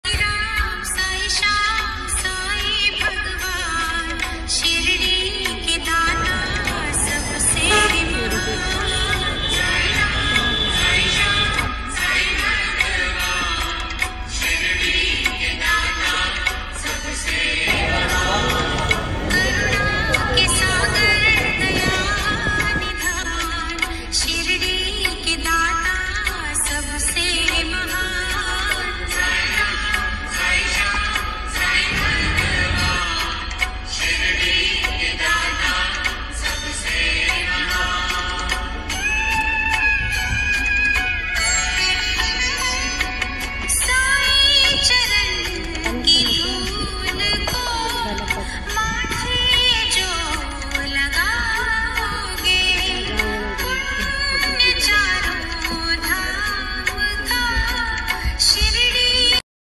God blessings to all - Family visit to Sai Baba temple at Shiridi, Maharashtra, India - July 2023
God blessings to all - visit to Sai Baba temple at Shiridi, Maharashtra, India - July 2023